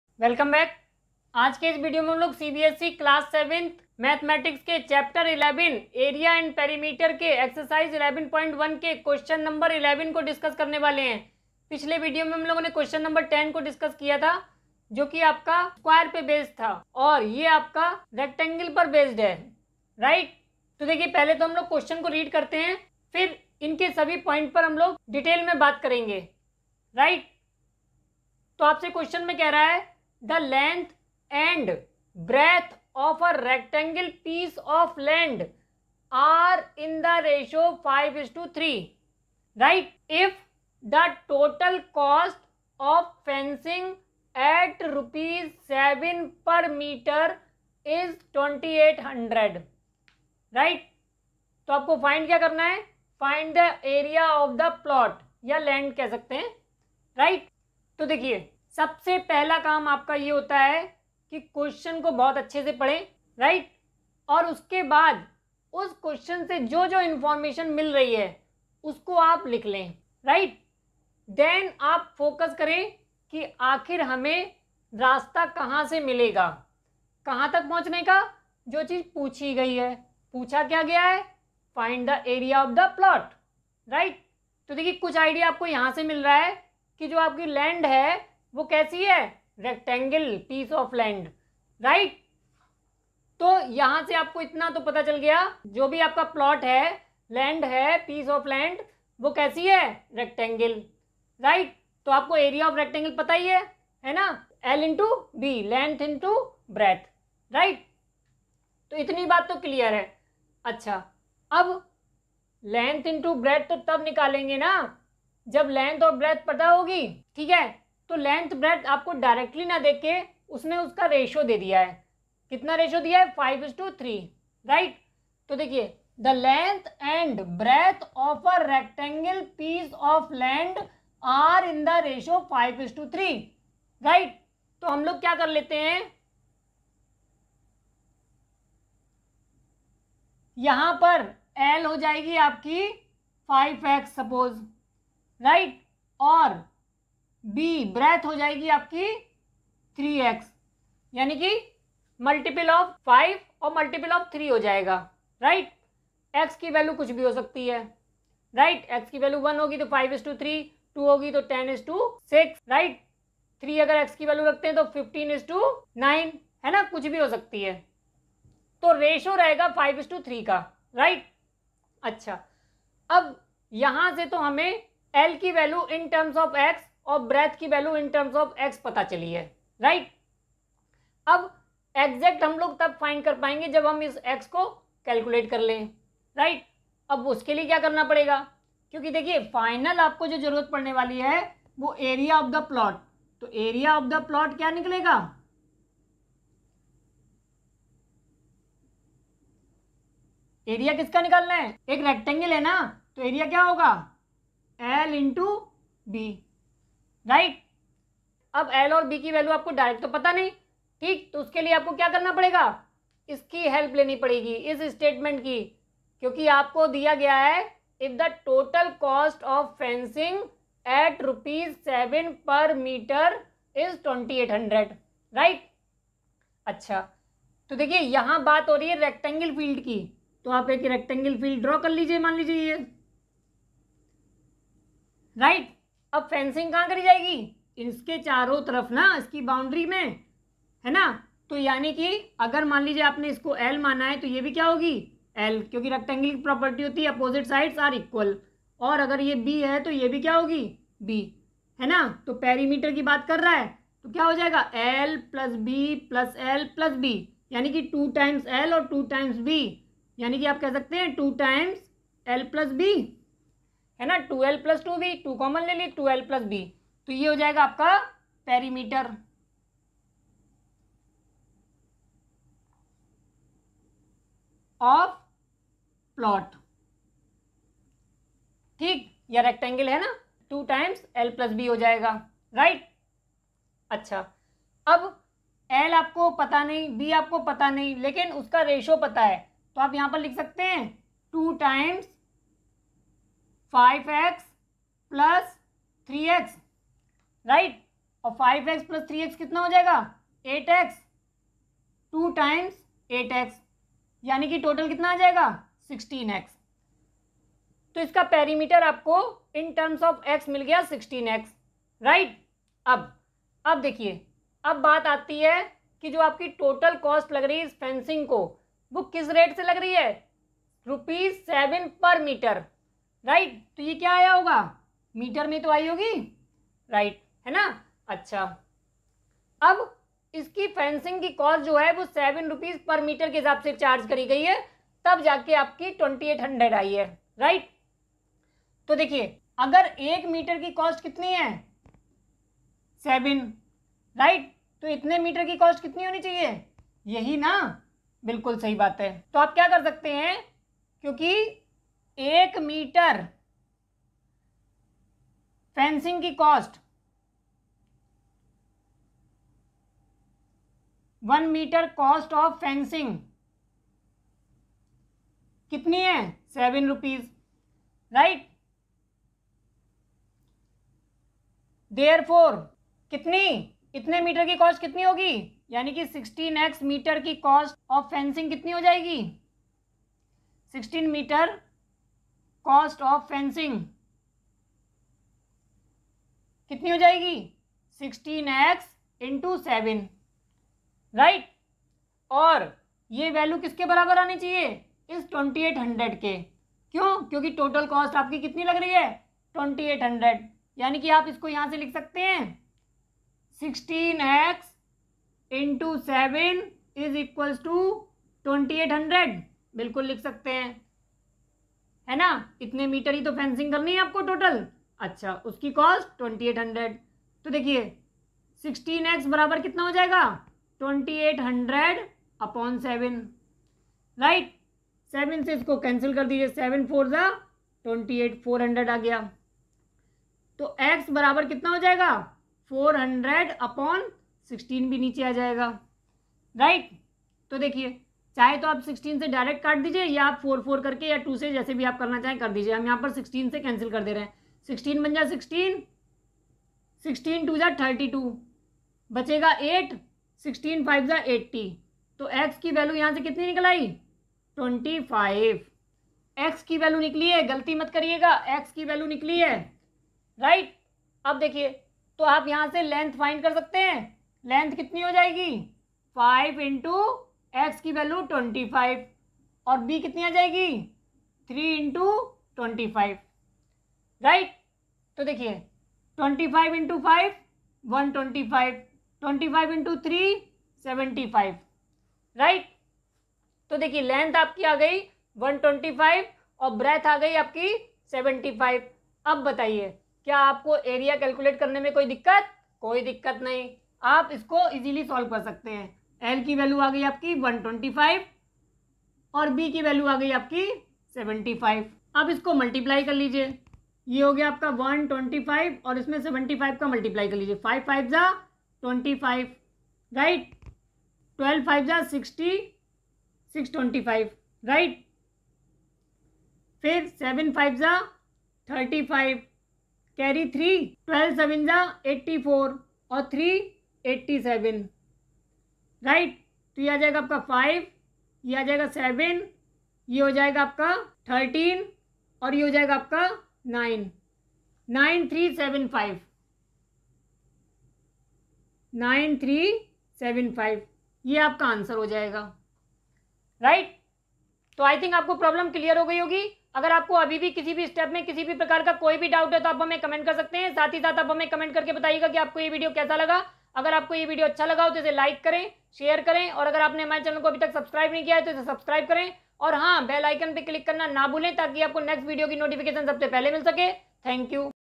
The length and breadth of a rectangle piece of land are in the ration 5:3. If total cost of fencing
The length and breadth of a rectangular piece of land are in the ratio 5:3. If the total cost of fencing at 7 Rs per meter is 2800. Find the area of the plot.

CBSE Class VII
Mathematics
Chapter-11: Perimeter and Area